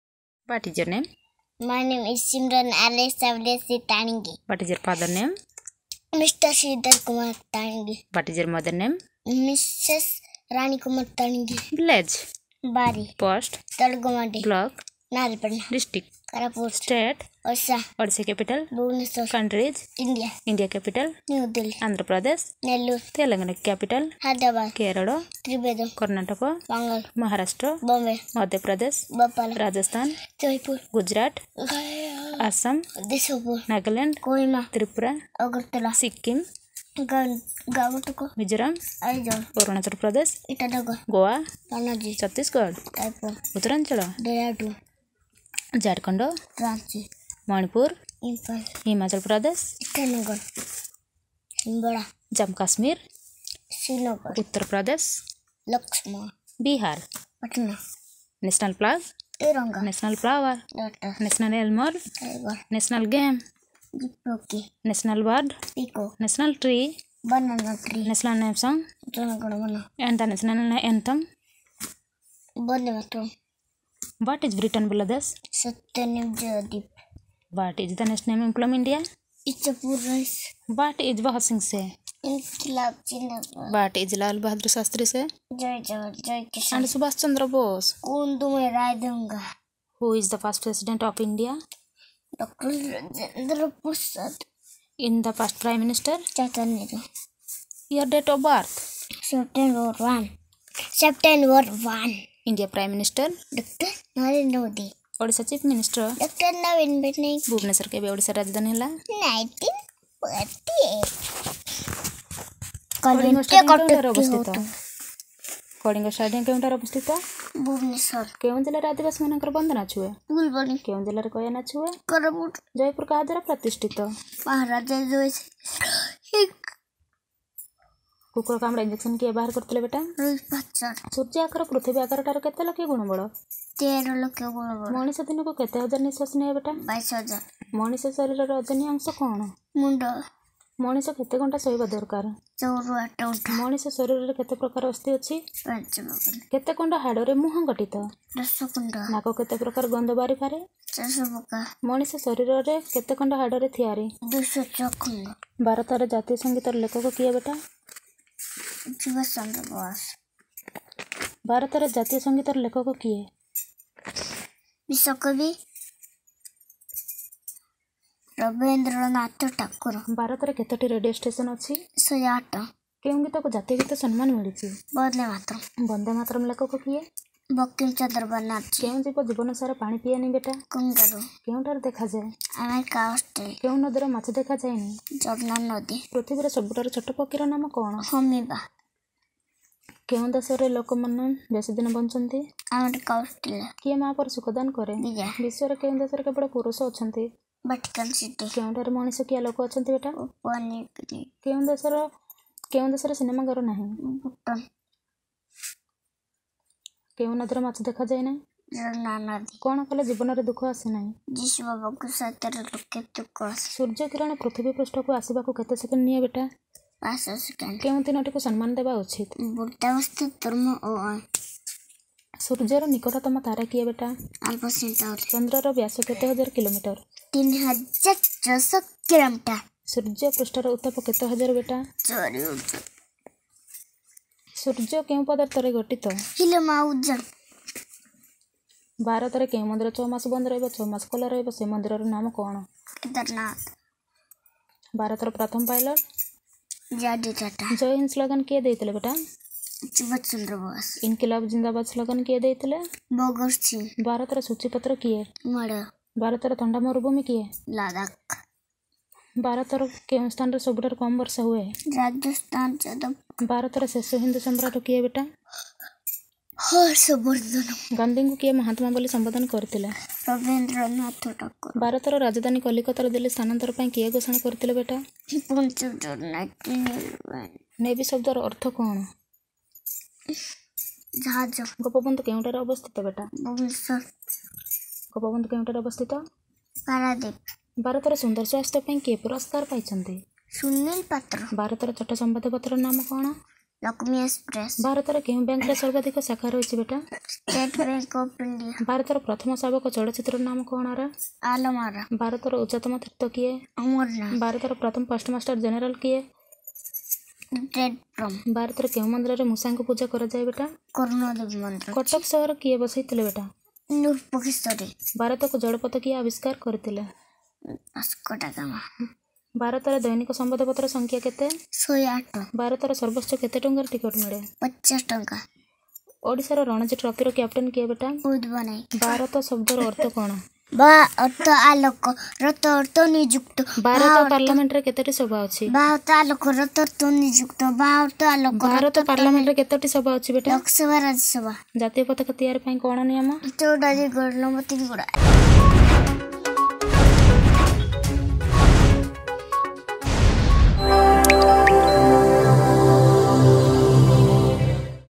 ଉପାନ୍ତ ଅଞ୍ଚଳର Google Girl ସିମ୍ରନ
କୋରାପୁଟ ଜିଲ୍ଲା ନାରାୟଣପାଟଣା ବ୍ଲକ ତଳଗୁମାଣ୍ଡି ପଂଚାୟତର ବାରୀ ଗ୍ରାମର ଶ୍ରୀଧର କୁମାର ତାଡିଙ୍ଗି ଙ୍କ ଦୁଇ ବର୍ଷର ଝିଅ ସିମ୍ରନ ପାଞ୍ଚ ମିନିଟ୍ ରେ ଦେଶ ଓ ରାଜ୍ୟର ସାଧାରଣ ଜ୍ଞାନ ସବୁ ଉତ୍ତର ଦେଉଛନ୍ତି ।